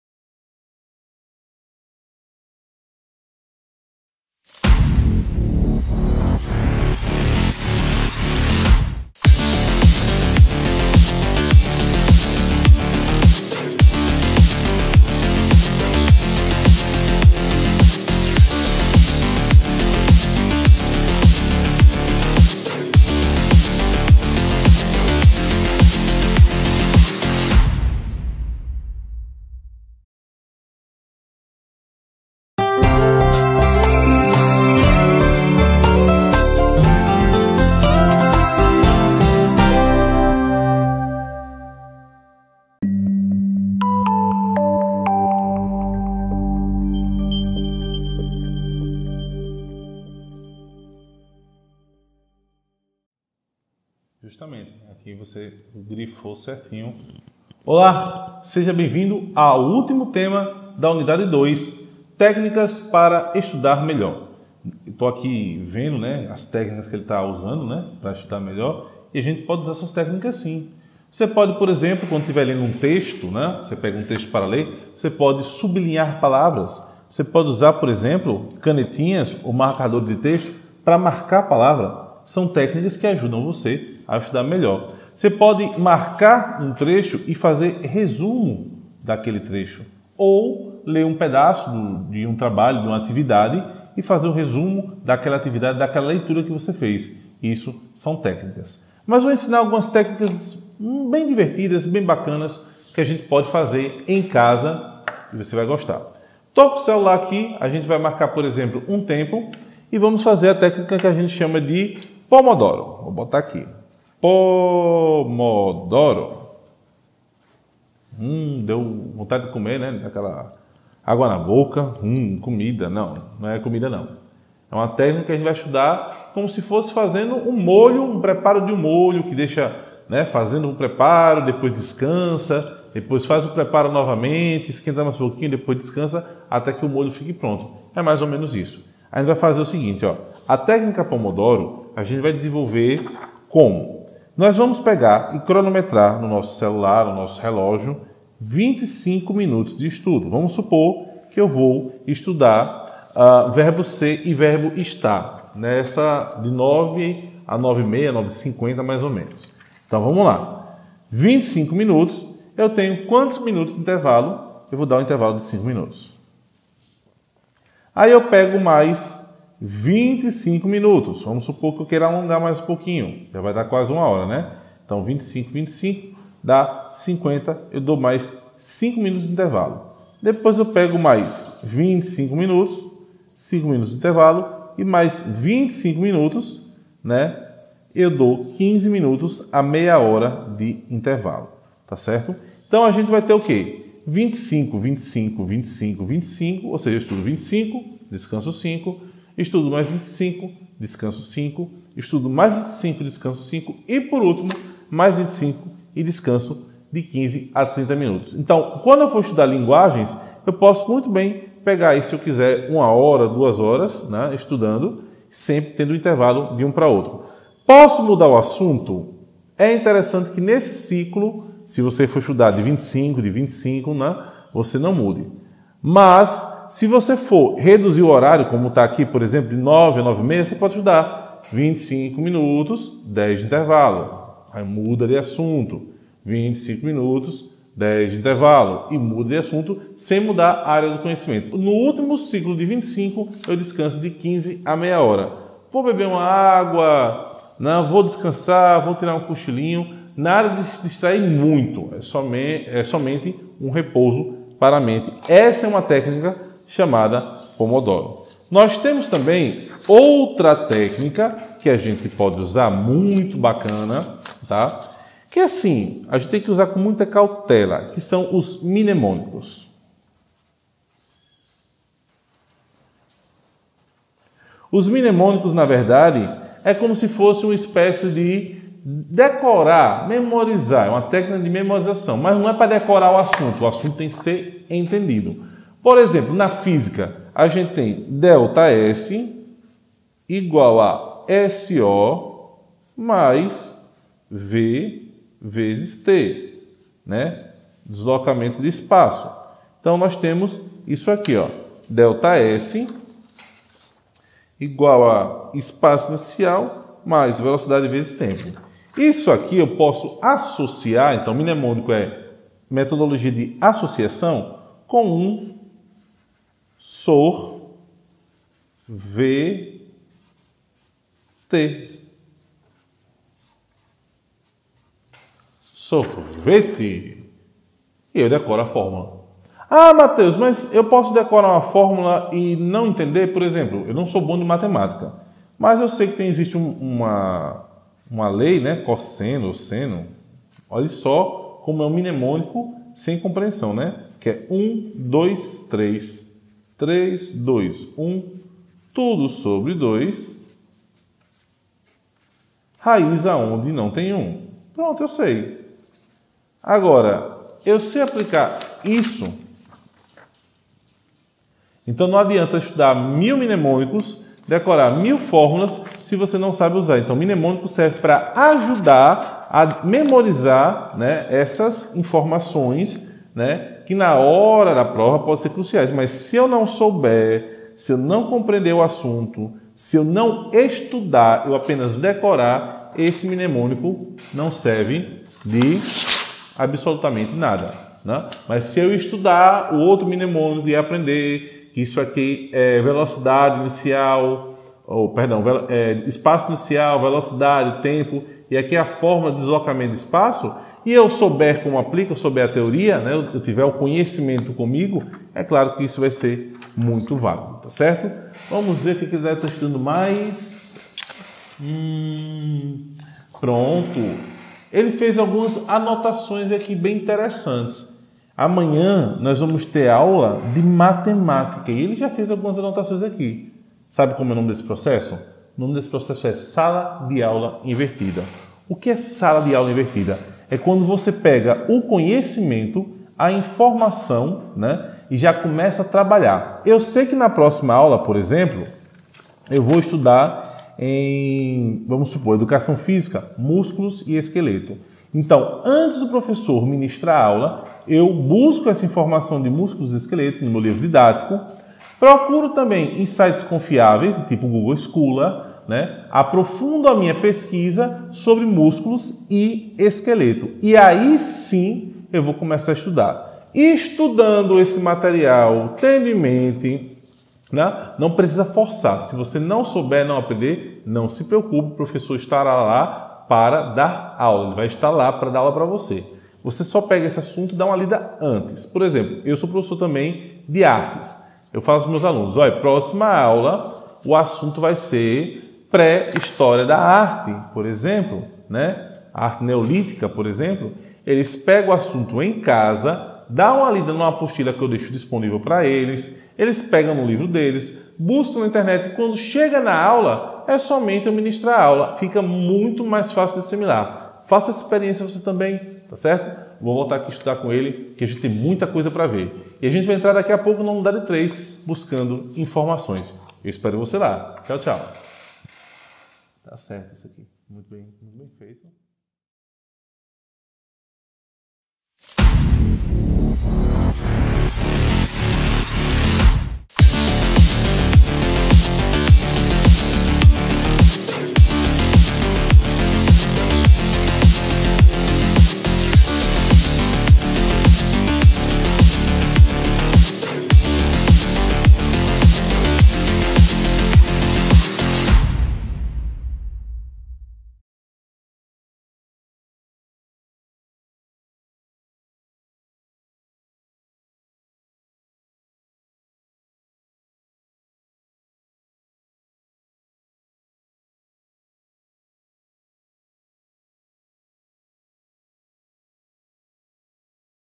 T07 - Técnicas para estudar melhor.
Esta é a aula referente ao Tema 07 do Unidade 02 do curso "Letramento Informacional: práticas e perspectivas para o Novo Ensino Médio".

Este trabalho é originário do Mestrado em Gestão da Informação e do Conhecimento oferecido pelo Departamento de Ciência da Informação da Universidade Federal de Sergipe (PPGCI/UFS).

Estarei à disposição para lhe ajudar!